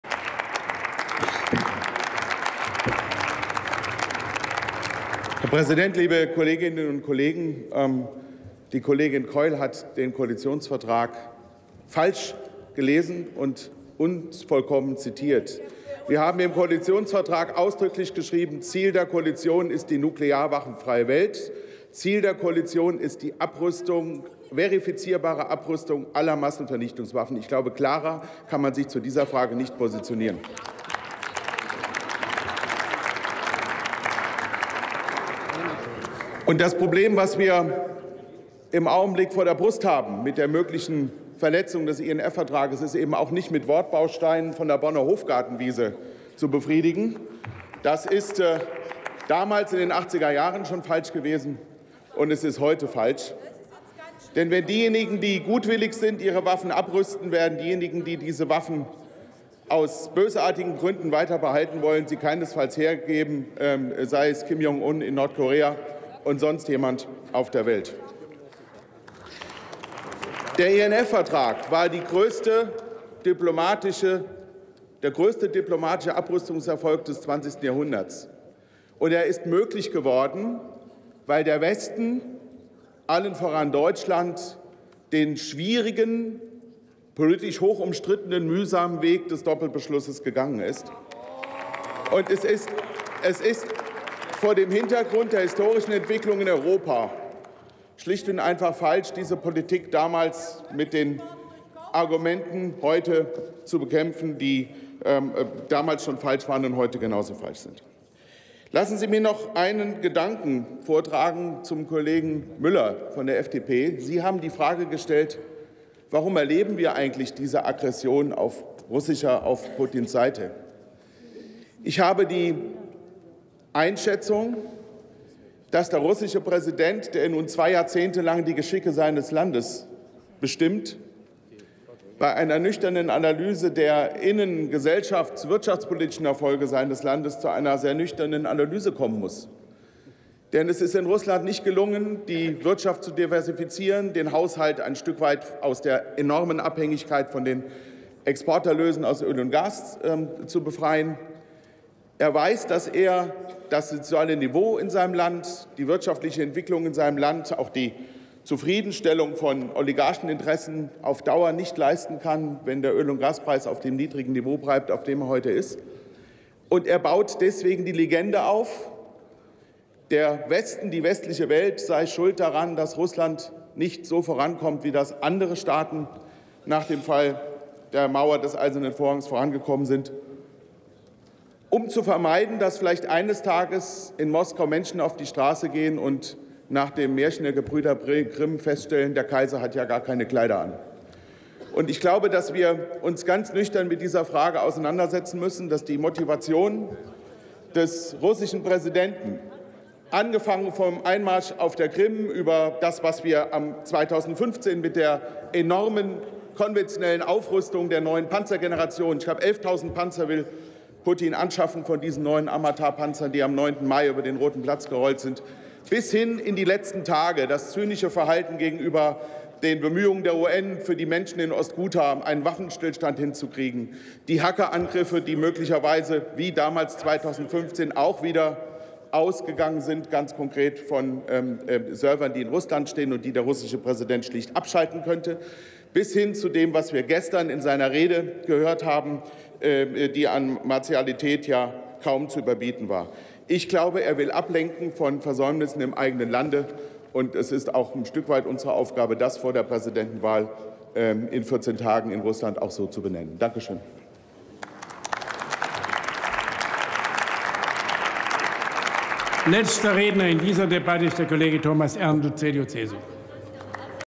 Jürgen Hardt: Abrüstungspolitik [Bundestag 02.03.2018]
a) Beratung des Antrags der Fraktionen der CDU/CSU und SPD
Den INF-Vertrag als Grundpfeiler atomarer Sicherheitsarchitektur und Kernelement europäischer Sicherheit erhalten
Drucksache 19/956

Glaubhafter Einsatz für Nukleare Abrüstung – Nationale Handlungsspielräume nutzen
Drucksache 19/976

18. Deutscher Bundestag
18. Plenarsitzung
Tagesordnungspunkt: Abrüstungspolitik
Redner: Hardt, Jürgen, CDU/CSU
Partei: Christlich Demokratische Union (CDU)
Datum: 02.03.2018
Plenarprotokoll:
UVID:7206195